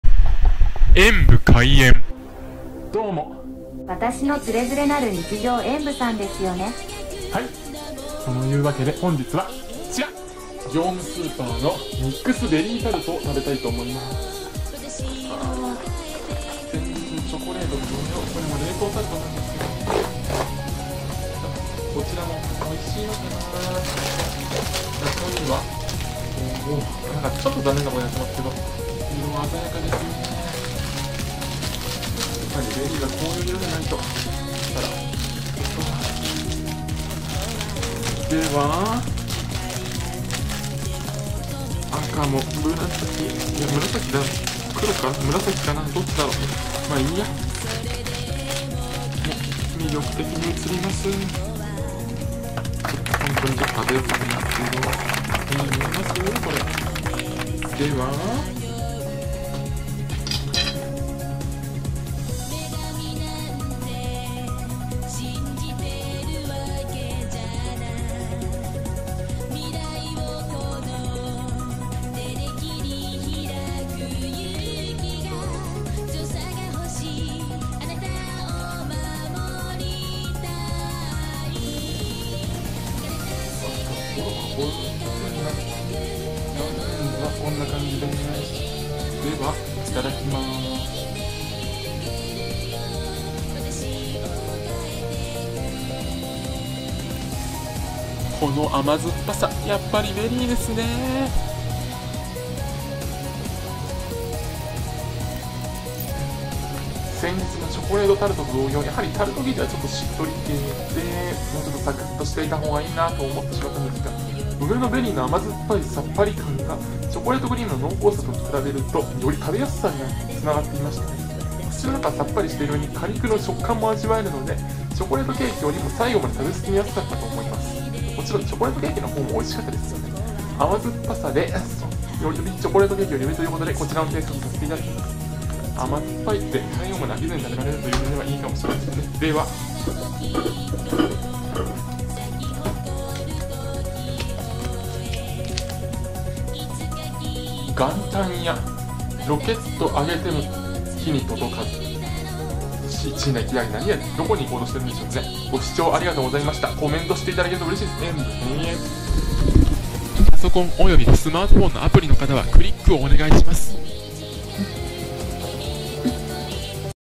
業務スーパー ミックスベリータルトを食べてみた。甘酸っぱさを堪能~
業務スーパーのミックスベリータルトを食べてみました。一面に敷き詰められたベリーが甘酸っぱさを堪能させてくれます。
ジュワっと広がりますよ～
贈り物は此方まで↓
〒153-0042